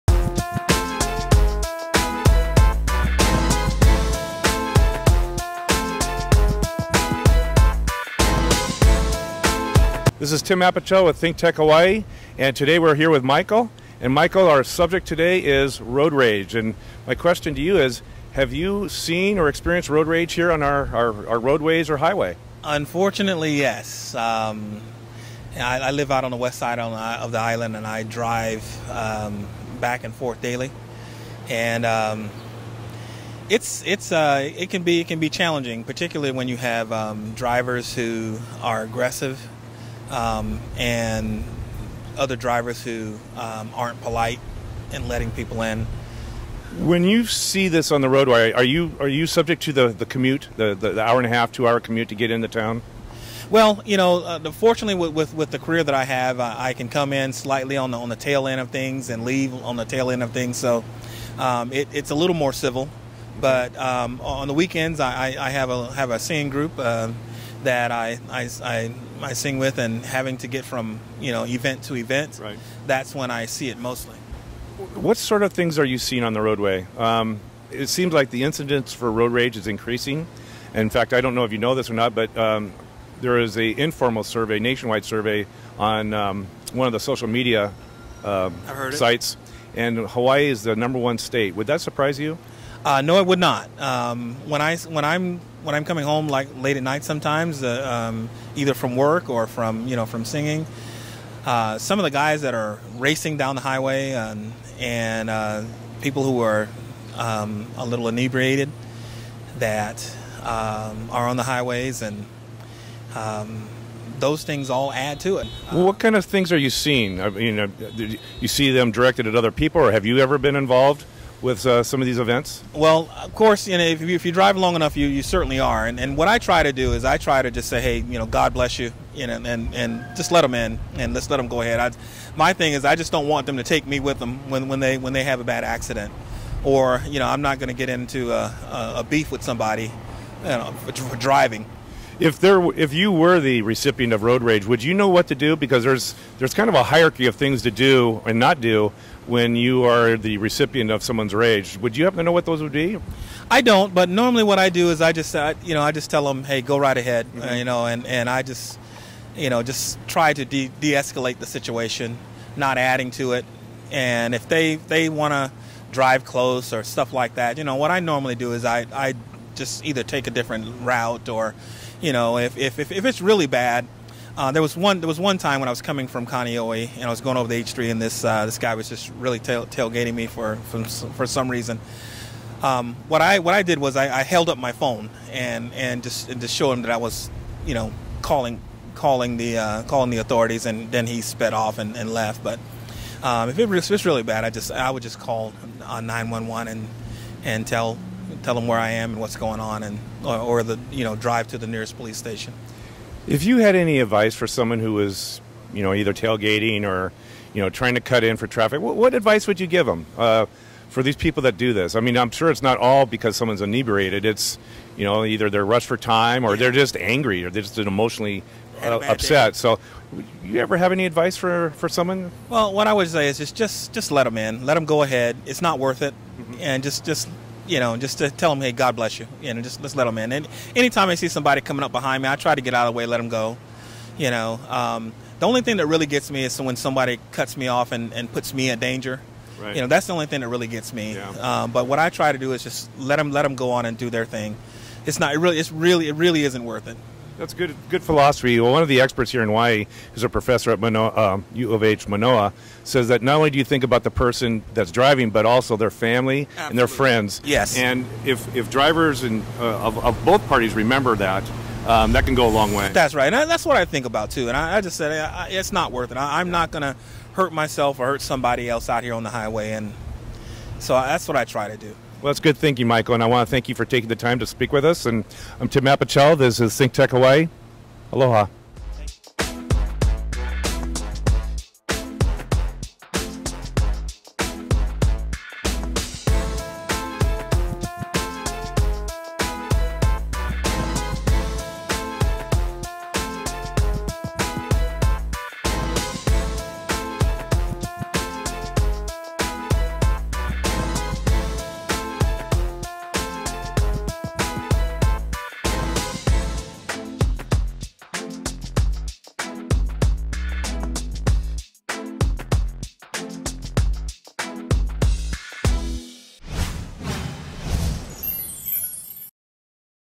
Have You Experienced Road Rage?
Moving Hawaii Forward Host, Tim Apicella talks with people on the street about thing thats matter to them, This week he tackles the topic of Road Rage in Hawaii.